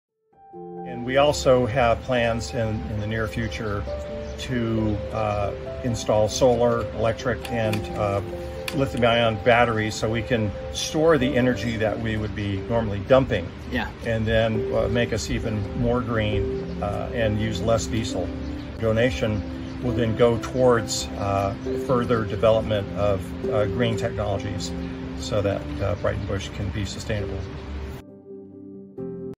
Breitenbush Solar and Green Technologies
Breitenbush also has plans to include solar power and lithium ion batteries to balance the energy production from the hydro-electric power and to continue moving toward greater sustainability and green technologies.